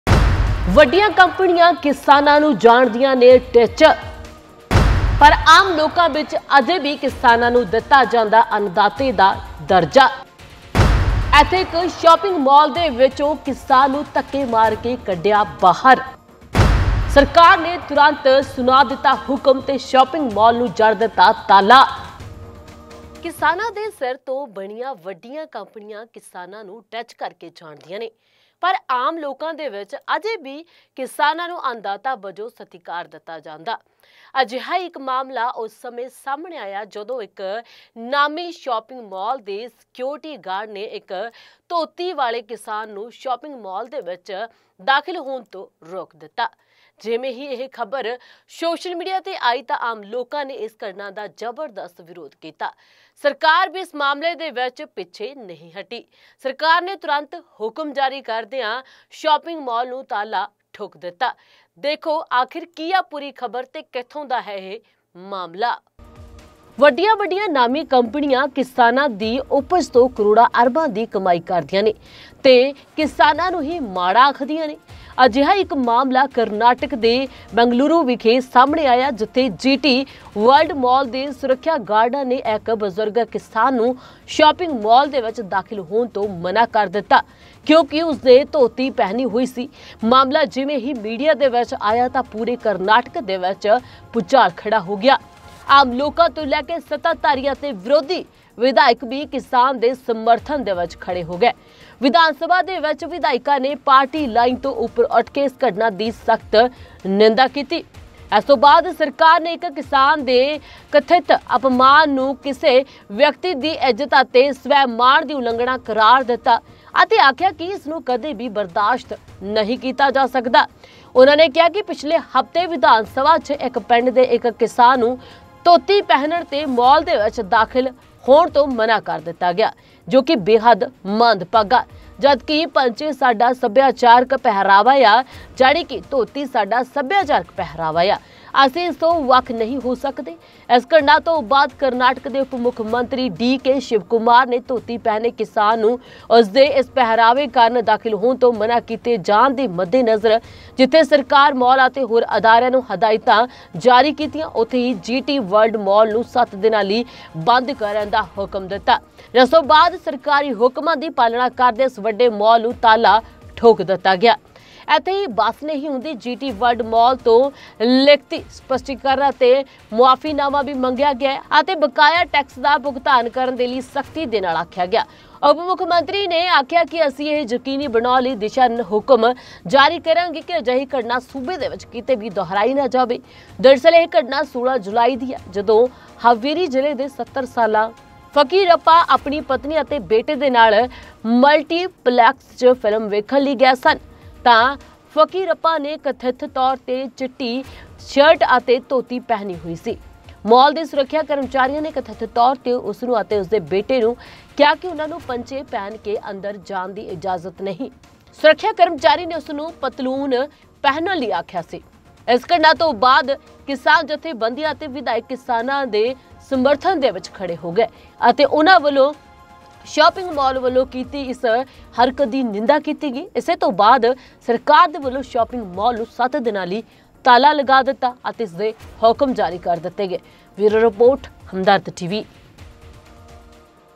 ਧੋਤੀ ਵਾਲੇ ਬਜ਼ੁਰਗ ਕਿਸਾਨ ਨੂੰ ਸ਼ਾਪਿੰਗ ਮਾਲ ’ਚ ਜਾਣੋਂ ਰੋਕਿਆ
ਪੈ ਗਿਆ ਗਾਹ, ਸਰਕਾਰ ਨੇ ਸ਼ਾਪਿੰਗ ਮਾਲ ਨੂੰ ਠੋਕਿਆ ਤਾਲਾ
ਇਕ ਕਿਸਾਨ ਦੇ ਸਮਰਥਨ ’ਚ ਪੂਰੀ ਸਟੇਟ ਹੋ ਗਈ ਖੜ੍ਹੀ